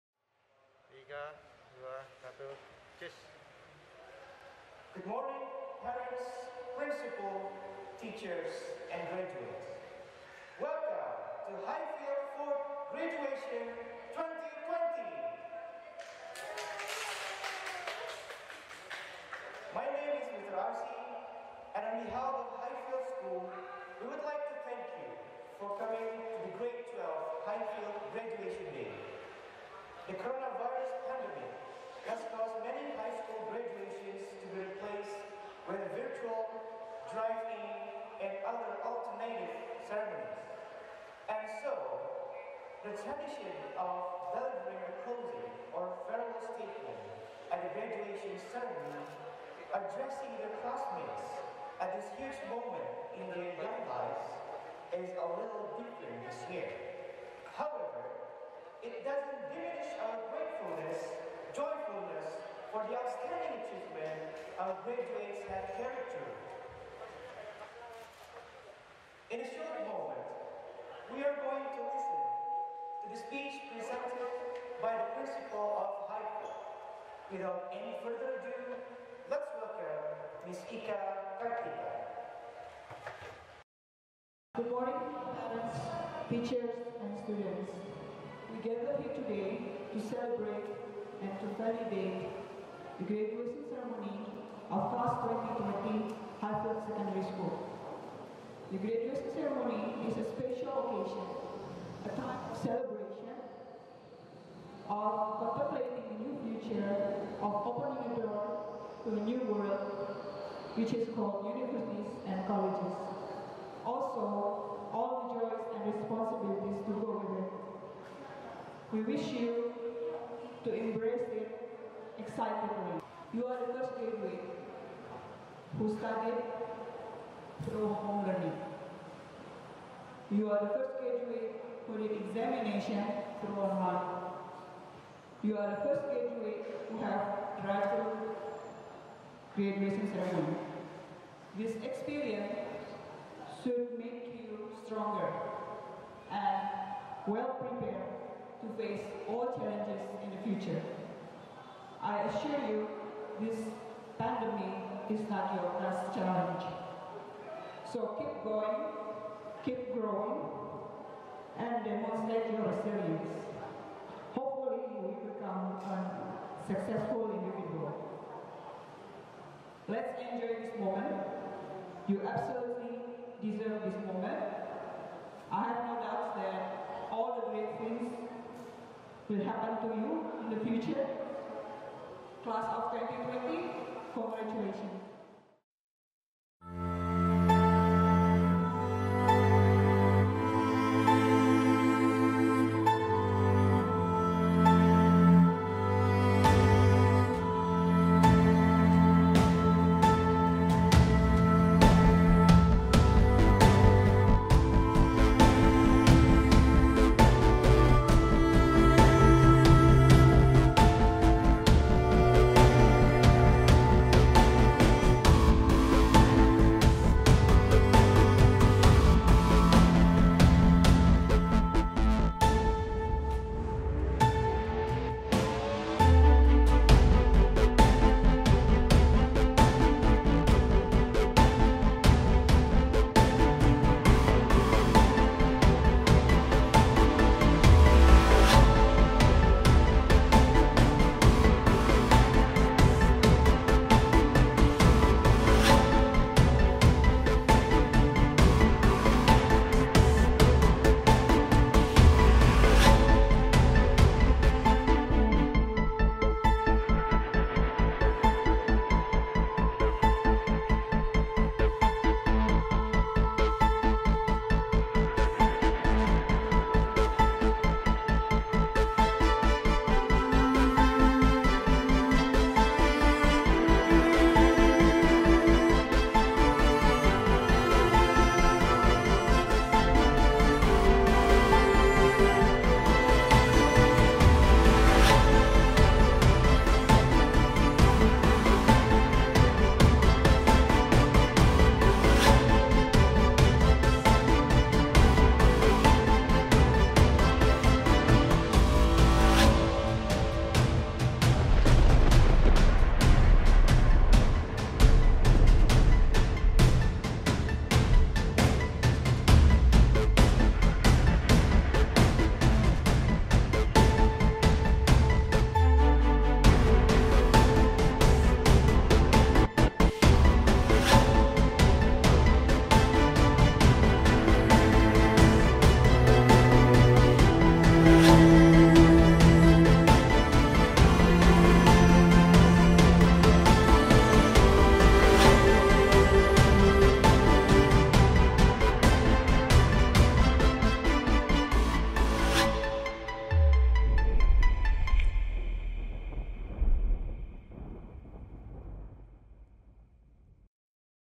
Highfield Graduation Class of 2020
July 11, 2020, the remarkable day for grade 12 students, officially graduated from Highfield Duren Sawit.

Due to global pandemic, the graduation ceremony was done uniquely, without reducing the happiness and harmony.

Let's congratulate them for their achievement accomplishing their senior highschool-life milestone and wish them the best for their higher education journey ahead.

Congrats graduates!